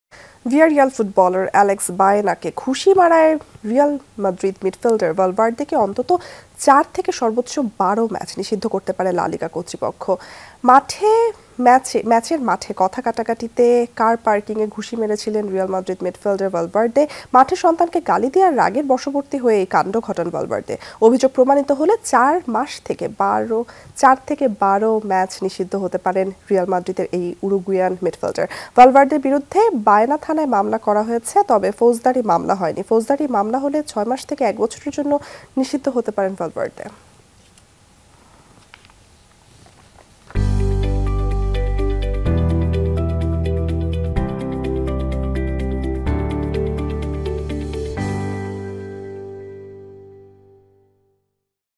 প্রতিপক্ষ খেলোয়াড়কে ঘুষি মারলেন রিয়ালের ভালভার্দে! | valbarde punch | Jamuna TV
ভিয়ারিয়াল ফুটবলার অ্যালেক্স বায়েনাকে ঘুষি মারায় রিয়াল মাদ্রিদ মিডফিল্ডার ভালভার্দেকে অন্তত ৪ থেকে সর্বোচ্চ ১২ ম্যাচ নিষিদ্ধ করতে পারে লা লিগা কর্তৃপক্ষ।